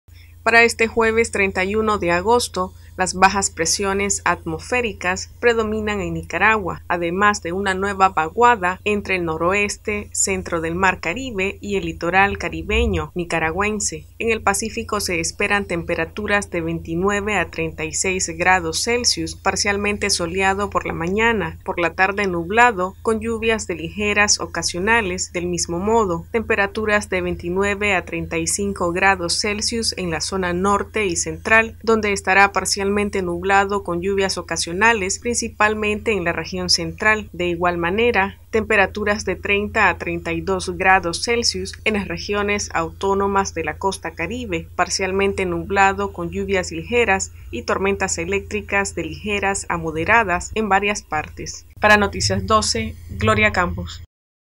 Noticias12Nicaragua Sinapred pronostica nublados y lluvias ocasionales
Sinapred pronostica nublados y lluvias ocasionales